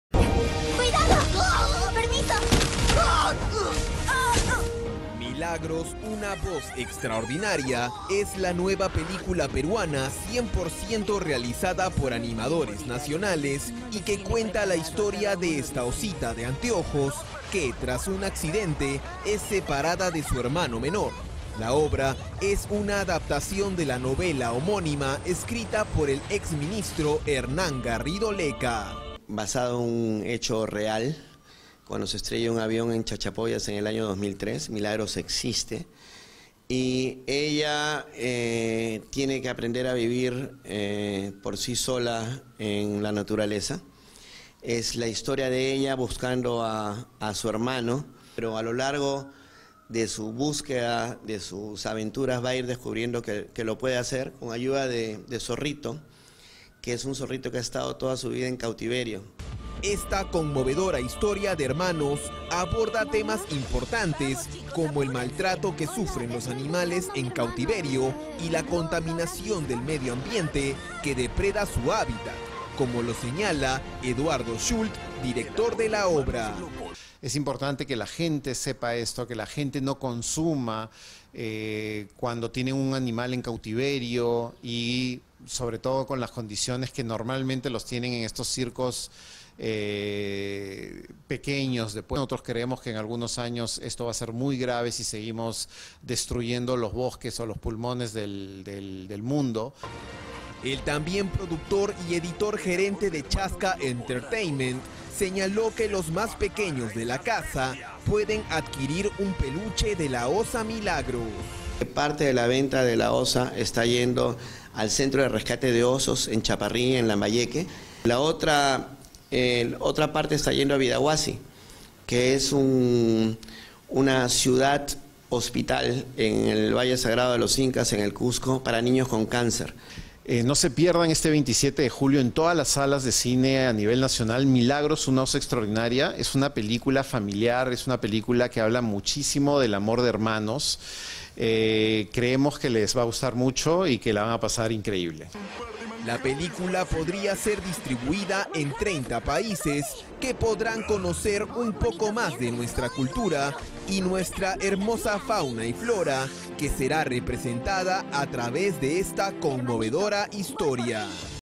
'Milagros: una osa extraordinaria', la cinta peruana que busca crear conciencia ambiental
&#39;Milagros: una osa extraordinaria&#39;, es la nueva película peruana que alista su estreno en la cartelera nacional. Esta cinta animada basada está en el libro del mismo nombre, escrita por el exministro Hernán Garrido-Lecca, que ganó el Premio Nacional Ambiental en el 2014.

El tráiler de la cinta realizada por Chaska Entertainment  fue estrenado en todas las plataformas virtuales y ha logrado gran expectativa para su próximo estreno. El filme esta dirigido a grandes y chicos.

La cinta está dirigida por el experimentado Eduardo Schuldt, que también realizó ‘Piratas en el Callao’ y ‘Dragones destinos de fuego’.

HISTORIA

La historia de esta película peruana tiene como protagonista a &#39;Milagros&#39;, una osa de anteojos con gran inteligencia y coraje que posee el don de entender el idioma humano y que enseñará a preservar y cuidar el medio ambiente.

Ella, junto a sus amigos, vivirán una y mil aventuras enfrentando los desafíos de la naturaleza y la crueldad del hombre para salvar a su hermano y preservar su especie.